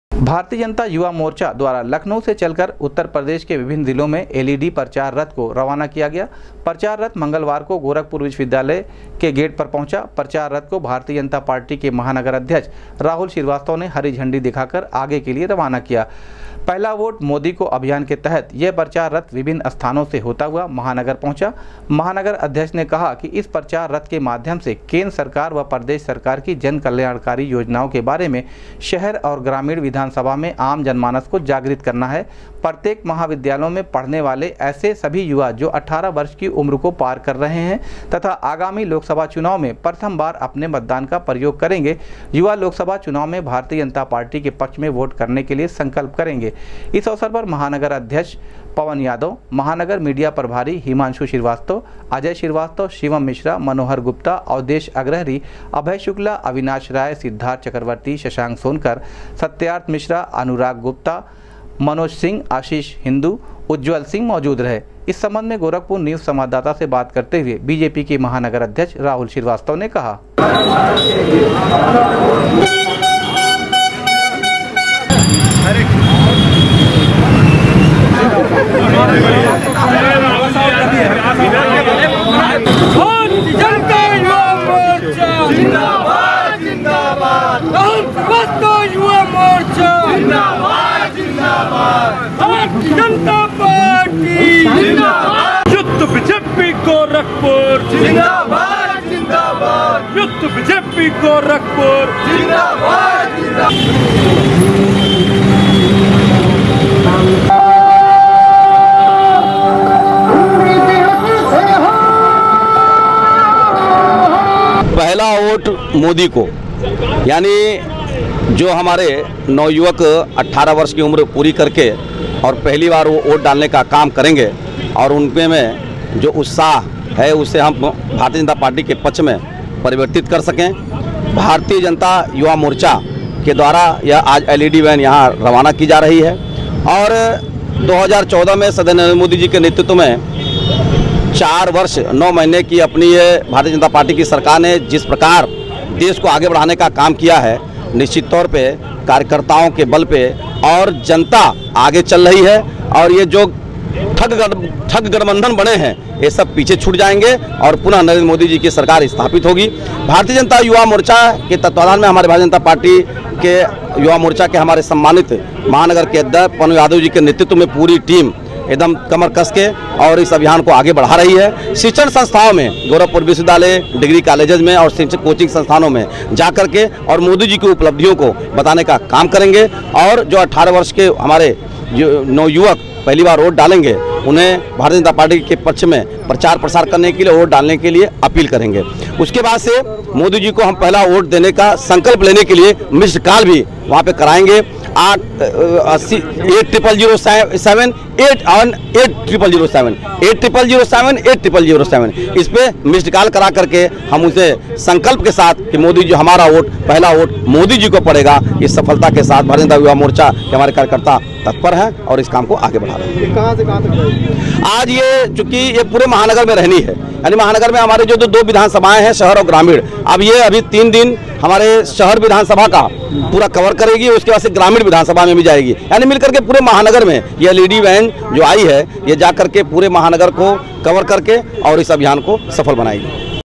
बीजेपी के महानगर अध्यक्ष ने एल0ई0डी0 प्रचार रथ को हरी झंडी दिखाकर किया रवाना, कहा पढ़ने
बीजेपी के महानगर अध्यक्ष ने एल0ई0डी0 प्रचार रथ को हरी झंडी दिखाकर किया रवाना, कहा पढ़ने वाले सभी 18 वर्ष की उम्र में वोट देने वाले मतदाता अपना पहला वोट दें मोदी को